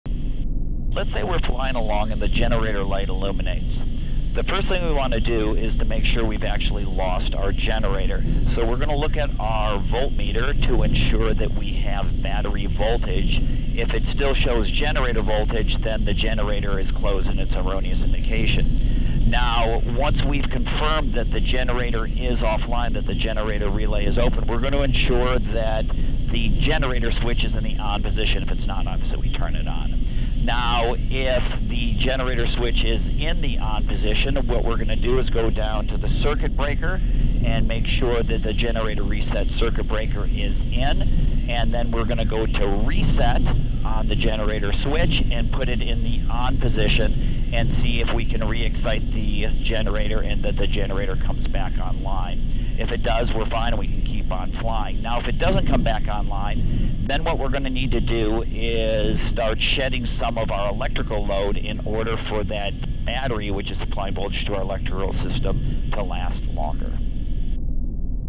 #222 - H130/H125 Airbus Helicopter Generator Failure Procedures
Knowledge is safety in the world of aviation.

Continuing Education

 Let's say we're flying along and the generator light illuminates. The first thing we want to do is to make sure we've actually lost our generator. So we're going to look at our voltmeter to ensure that we have battery voltage. If it still shows generator voltage, then the generator is closed and it's erroneous indication.

Now, once we've confirmed that the generator is offline, that the generator relay is open, we're going to ensure that the generator switch is in the on position. If it's not on, so we turn it on. Now, if If the generator switch is in the on position, what we're going to do is go down to the circuit breaker and make sure that the generator reset circuit breaker is in, and then we're going to go to reset on the generator switch and put it in the on position and see if we can re excite the generator and that the generator comes back online.

If it does, we're fine and we can keep on flying. Now, if it doesn't come back online, then what we're going to need to do is start shedding some of our electrical load. in order for that battery, which is applying voltage to our electrical system, to last longer.

IF THE BATTERY FAILS, THE VEMD WILL GO OUT AND NR/N2 INDICATION IS LOST.
APPLY THE PROCEDURE FOR FAILURE OF BOTH SCREENS (SECTION 3.5 § 1 VEMD SCREEN FAILURE).

AVOID AUTOROTATION, PERFORM A SHALLOW APPROACH WITH A CAUTIOUS LANDING.